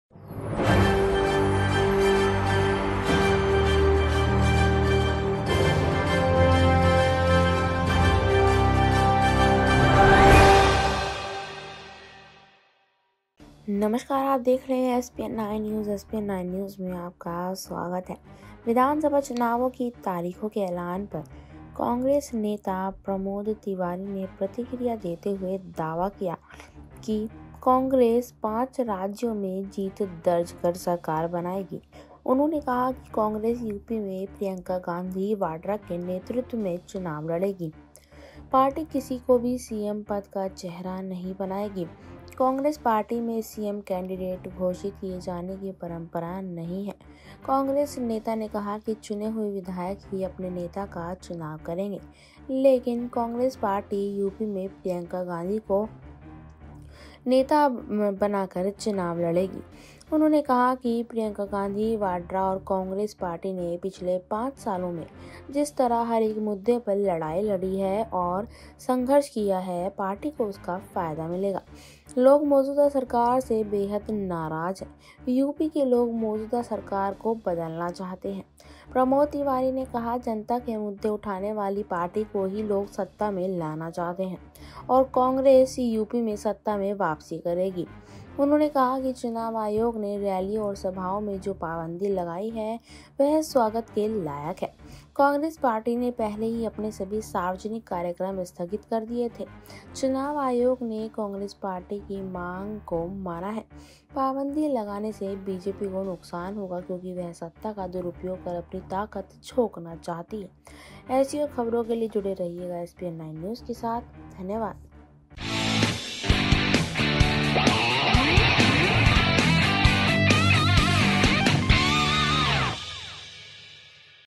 चुनावों की तारीख की घोषणा होने के बाद कांग्रेस ने भी कर दी एक नई घोषणा,कांग्रेस में मन जश्न...!!
चुनावों की तारीखों की घोषणा होने के बाद कांग्रेस ने भी की एक बहुत बड़ी घोषणा, इसी के साथ मन रहा है कांग्रेस में जश्न आगे की खबर जानने के लिए देखे ये विडियो |